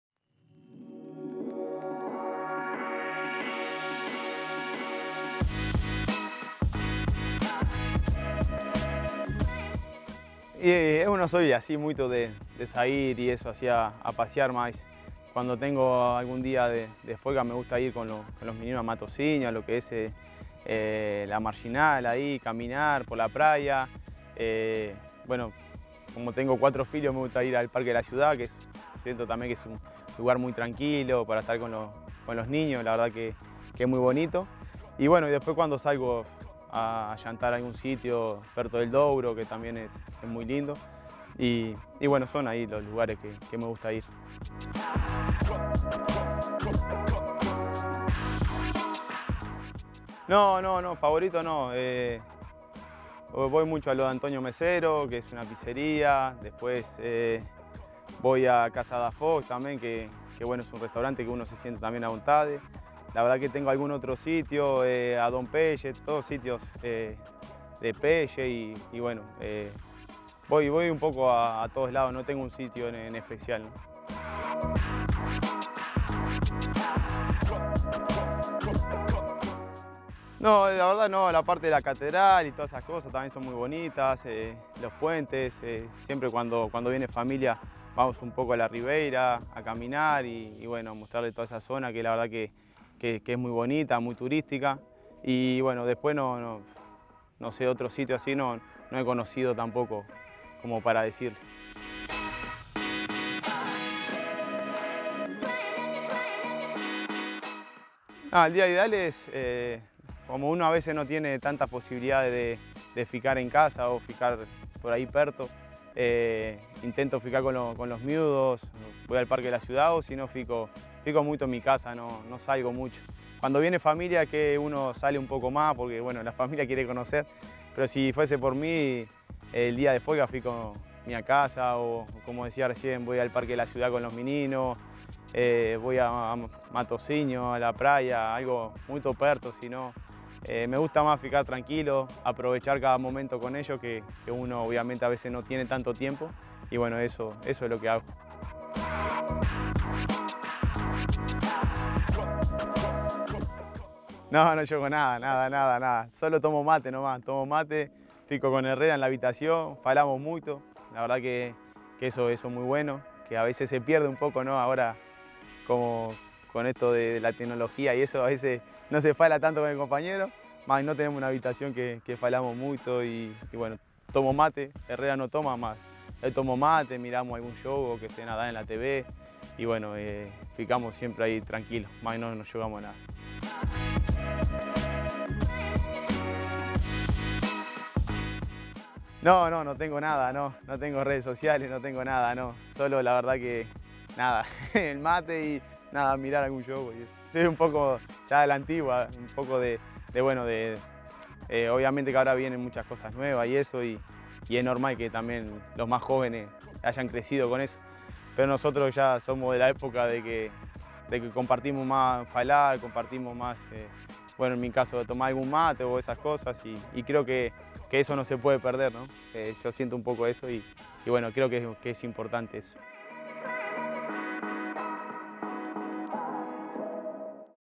Maxi Pereira: respostas rápidas
Lateral do FC Porto em entrevista à revista Dragões